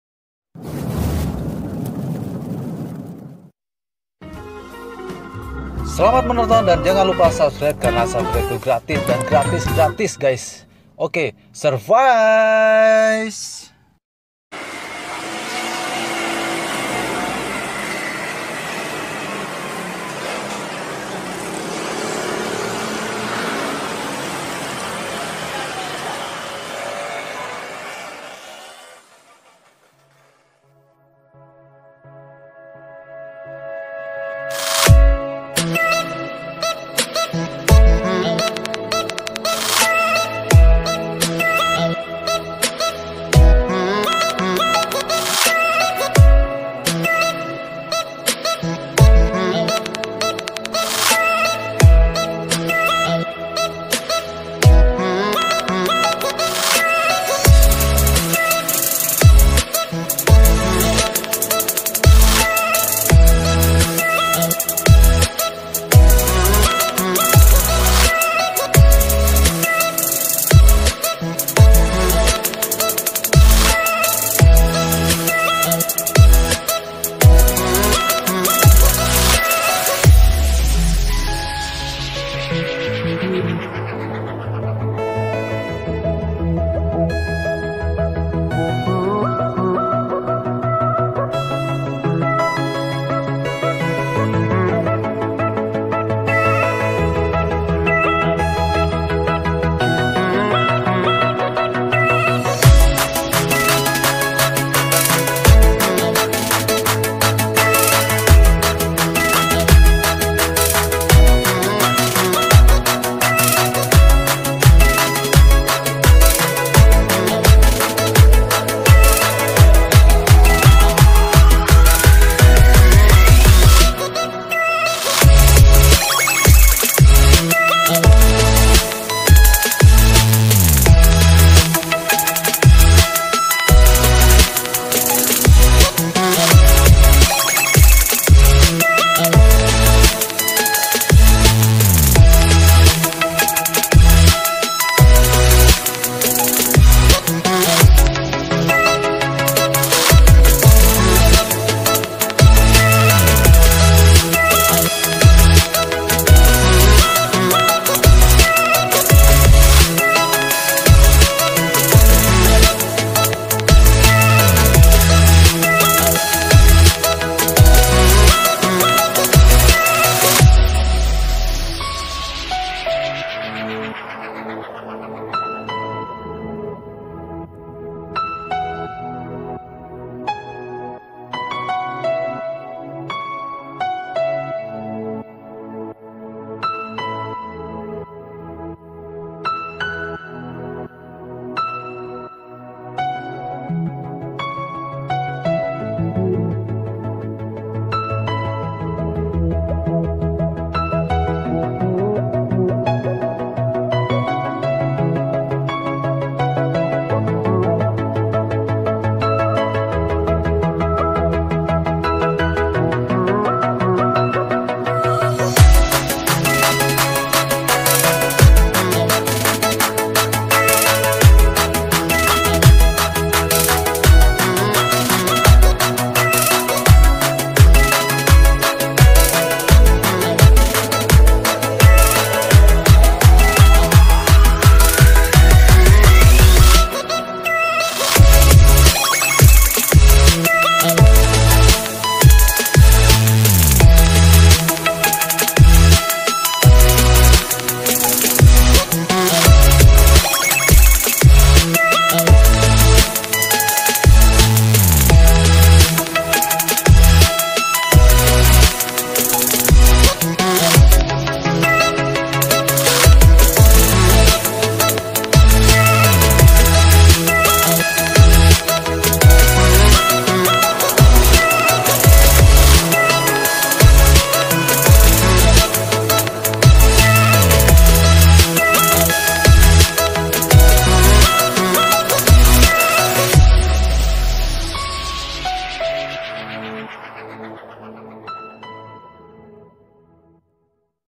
Pasang kaca film silver ATT tolak panas
Proses pemasangan kaca film att silver
Pasang blok kaca depan mengunakan att silver bisa memantulkan cahaya panas agar tidak terlalu panas. Selamat menonton dan selamat mencoba semoga video ini bermangfa'at untuk baraya semua.
Kami dari team Aden Variasi mengucapkan semoga sehat selalu dalam atifitas nya aminnnnn.
FB autodidack modifikasion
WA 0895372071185